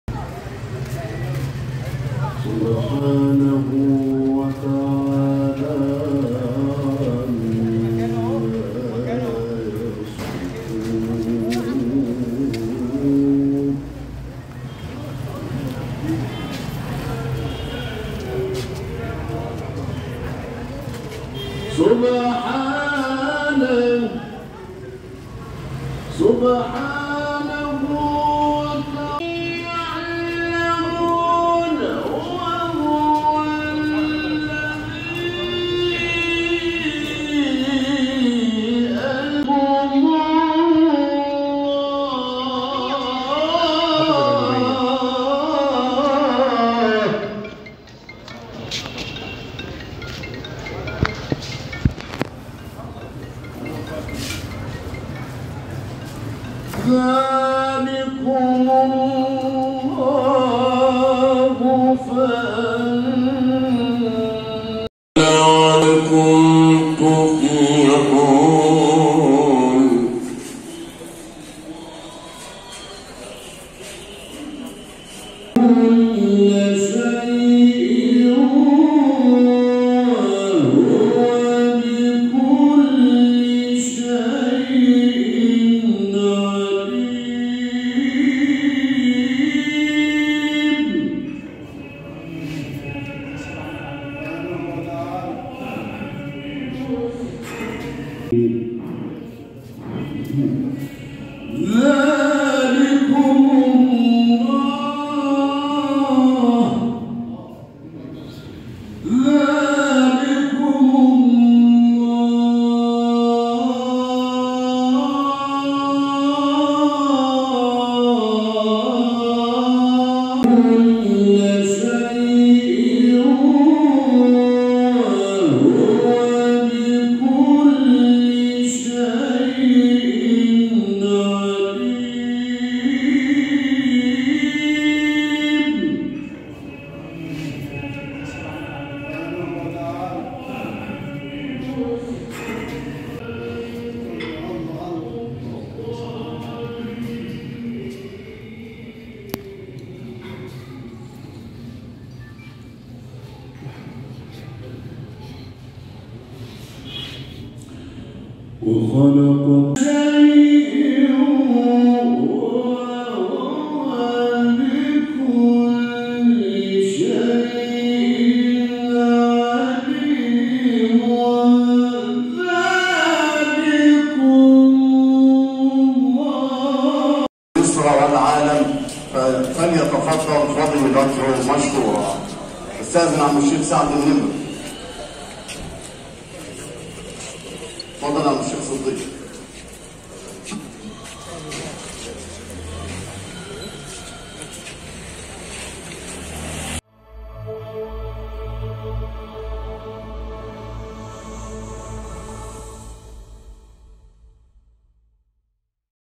كبار الشيوخ ورجال الدين في عزاء نجل عبد الباسط عبد الصمد
ElBaladOfficial#
ElBalad_Official#